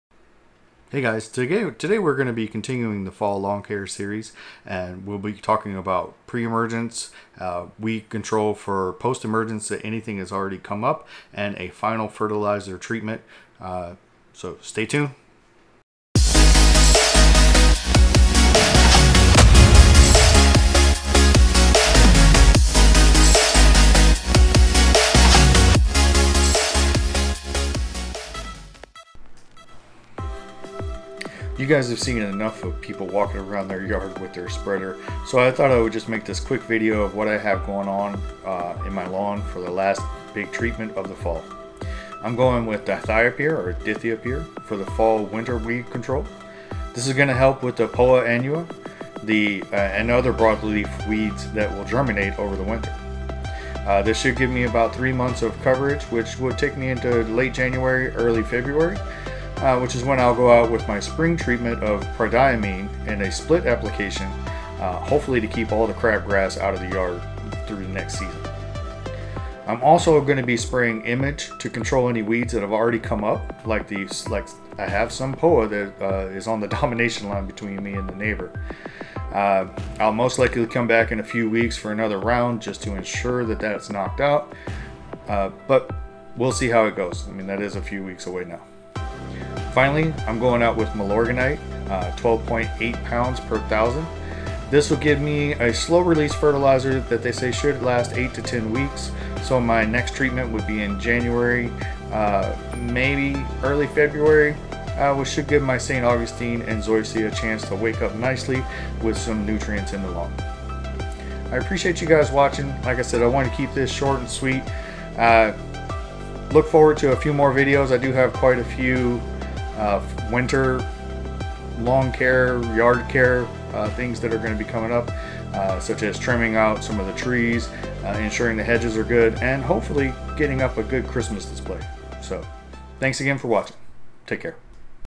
Fall Weed Control and Last Fertilizer of the Year - 120 Seconds
Really short video to give you some final tips on Fall lawn care as we get close to winter.  Pre Emergent, Post Emergent and Organic Fertilizer, how do you control your winter weeds so that you enter spring ready for a great season?

Long Intro but once you get to the whiteboard, the information is pure solid gold

Milorganite - Home Depot is where I got mine
Image - Picked up at Lowes